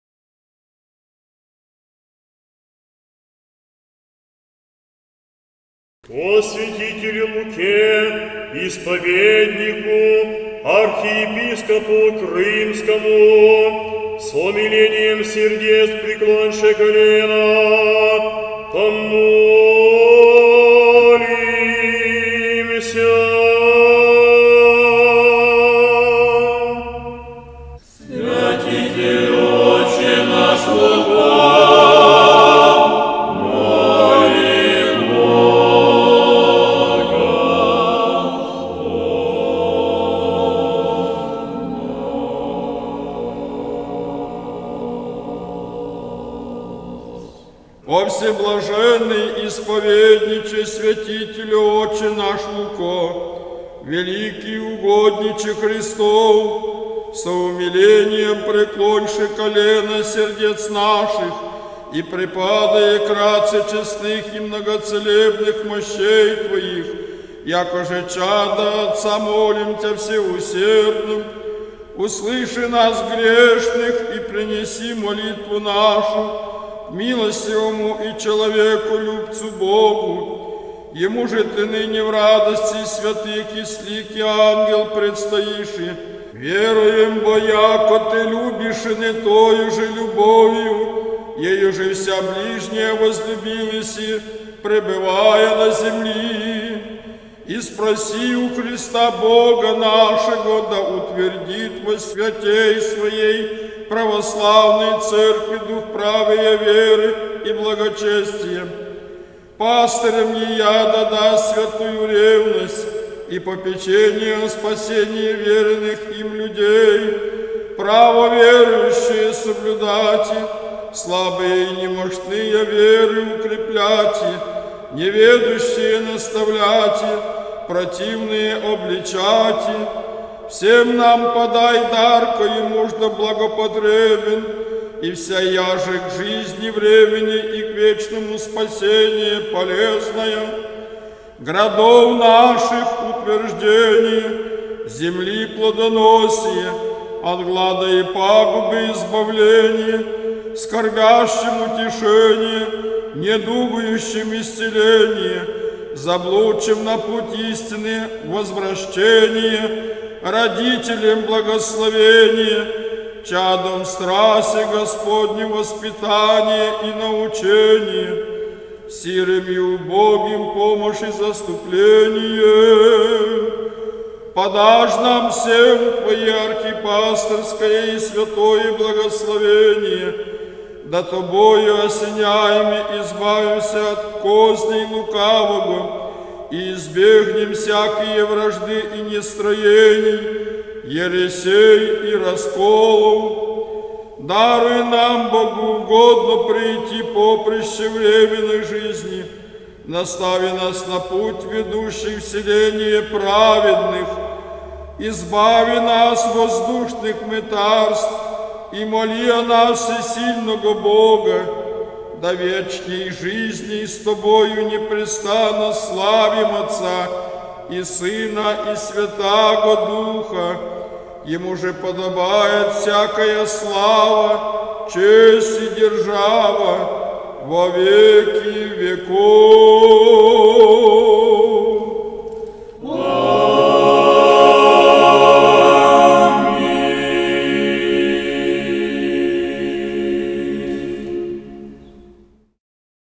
Молитва об исцелении от болезней Луке Крымскому
Если вы заболели и не знаете, что делать — обратитесь с молитвой об исцелении к Луке Крымскому .

Молитва о здоровье и исцелении больных на протяжении вот уже многих столетий остается одной из наиболее сильных и эффективных. Перед началом лечения следует обязательно прочитать молитву и очистить свою душу от негатива, который может помешать вам исцелиться.

Текст молитвы об исцелении:
О всеблаженный исповедниче, святителю отче наш Луко, великий угодниче Христов. Со умилением приклоньше колена сердец наших, и припадая к раце честных и многоцелебных мощей твоих, якоже чада отца молим тя всеусердно: услыши нас, грешных, и принеси молитву нашу к милостивому и человеколюбивому Богу, Емуже ты ныне в радости святых и с лики ангел предстоиши. Веруем бо, яко ты любиши ны тою же любовию еюже вся ближния возлюбил еси, пребывая на земли.

Испроси у Христа Бога нашего, да утвердит чад Своих в духе правыя веры и благочестия: пастырям да даст святую ревность и попечение о спасении вверенных им людей: право верующия соблюдати, слабые и немощныя в вере укрепляти, неведущия наставляти, противныя обличати.

Всем нам подай дар коемуждо благопотребен, и вся яже к жизни временней и к вечному спасению полезная: градов наших утверждение, земли плодоносие, от глада и пагубы избавление, скорбящим утешение, недугующим исцеление, заблудшим на путь истины возвращение, родителем благословение, чадом в страсе Господнем воспитание и научение, сирым и убогим помощь и заступление.

Подаждь нам всем твое Архипастырское благословение, да таковое молитвенное ходатайство имущи, избавимся от козней лукаваго и избегнем всякия вражды и нестроений, ересей и расколов. Настави нас на путь, ведущий в селения праведных, и моли о нас всесильнаго Бога, в вечней жизни сподобимся с тобою непрестанно славити Единосущную и Нераздельную Троицу, Отца и Сына и Святаго Духа. Аминь.